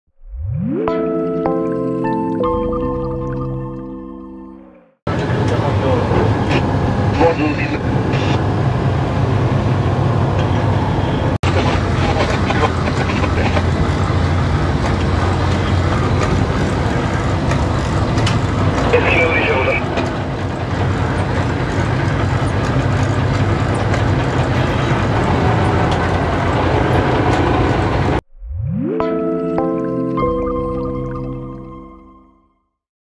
Barca a vela naufraga in Sicilia: la Guardia costiera salva due persone
Video: Guardia Costiera